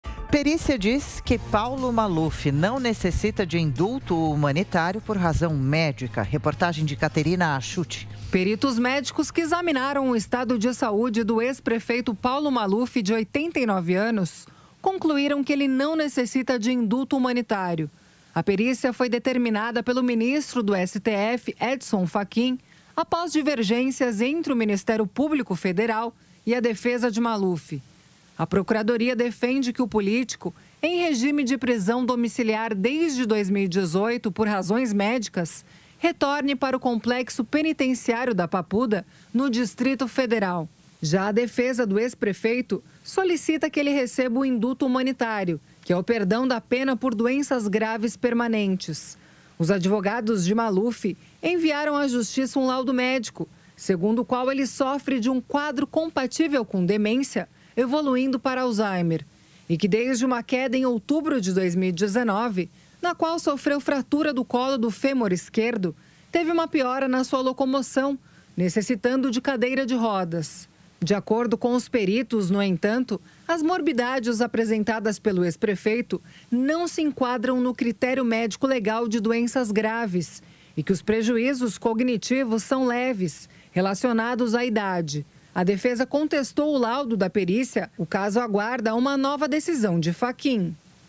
Perícia diz que Paulo Maluf não necessita de indulto por razão médica
Peritos médicos que examinaram o estado de saúde do ex-prefeito, Paulo Maluf, de 89 anos, concluíram que ele não necessita de indulto humanitário. A perícia foi determinada pelo ministro do STF, Edson Fachin, após divergências entre o MPF e a defesa de Maluf.